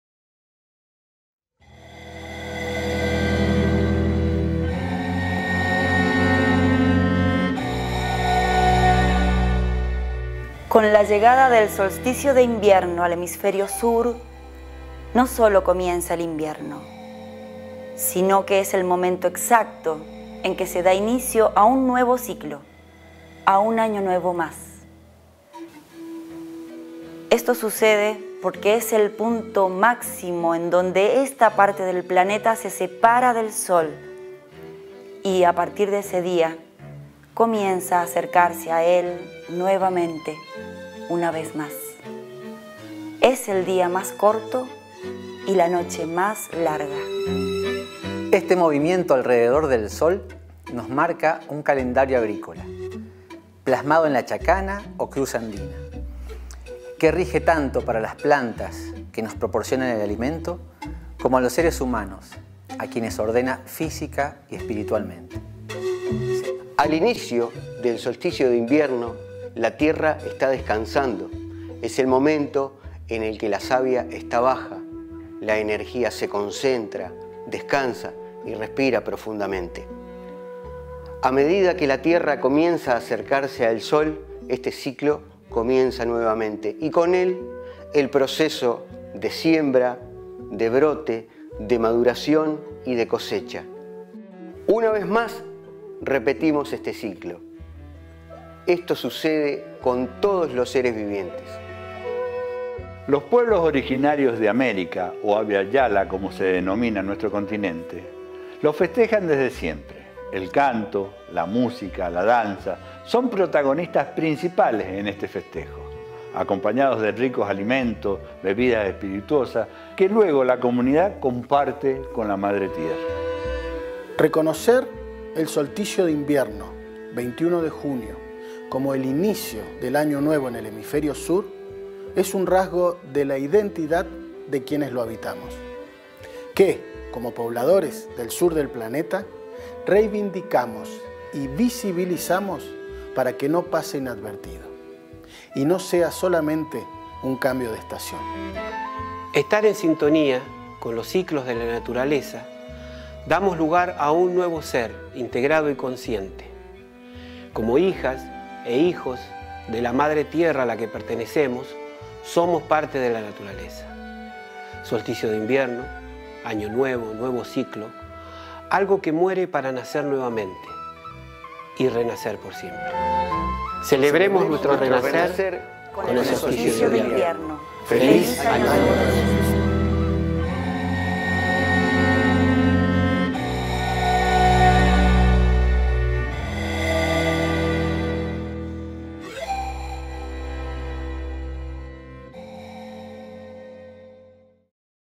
MANIFIESTO Año Nuevo del Sur - Patàn Purpora
Porquè tenemos que festejar el Año Nuevo en junio, los que vivimos al Sur del Ecuador??